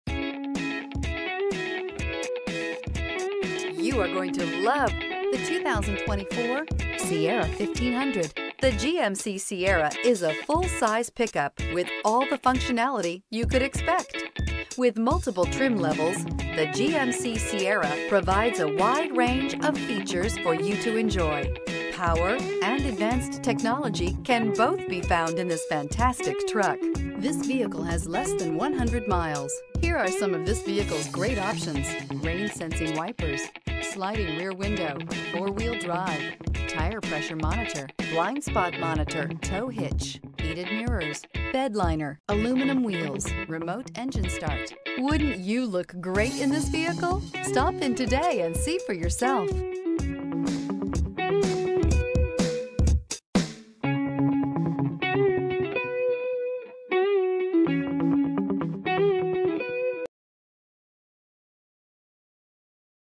2024 GMC Sierra 1500 D24273
2024 GMC Sierra 1500 Denali

2024 GMC Sierra 1500 Denali 10-Speed Automatic, 4WD, Jet Black Leather. Denali23/27 City/Highway MPG